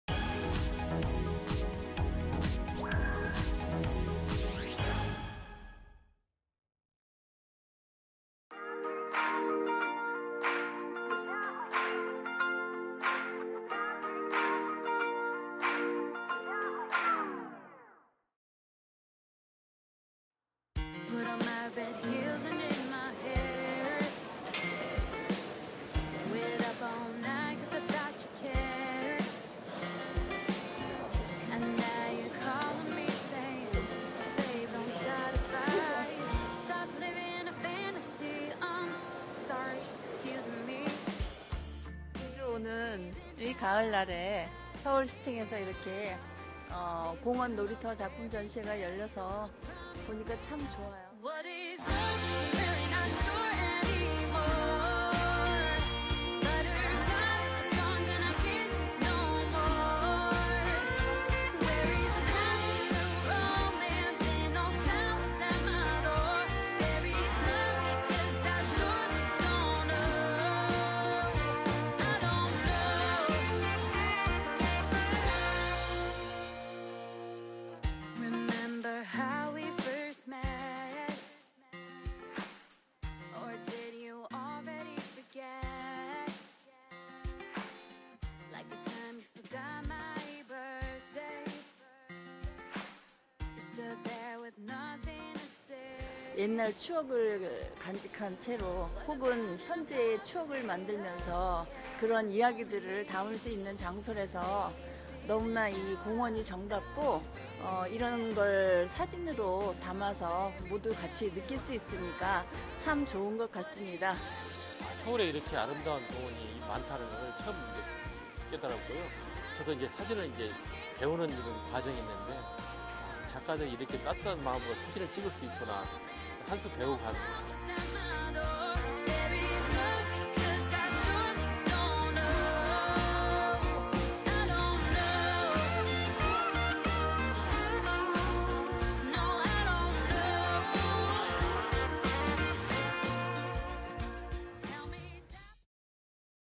시민, 서울의 공원을 담다 [tbs현장렉]
[tbs현장렉] 시민, 서울의 공원을 담다

서울시 공원사진사들의 이색 사진전인 '공원은 놀이터'가 개최되었습니다!
공원사진사로 선발된 72명 작가가 공원에서 여가를 즐기는 시민들을 촬영했는데요.
도시공원의 새로운 의미와 놀이터로서의 가치를 공유하고자 마련된 자리입니다.
공원을 찾은 아이들과 어른들, 그 찰나의 순간들을 담은 사진들을 볼 수 있는데요.
서울시의 다양한 공원의 모습과 사진을 통해 공원에서의 추억을 나눌 수 있습니다.
공원에서의 따뜻한 일상 속 이야기!
이번 주말 '공원은 놀이터' 사진전은 어떠세요?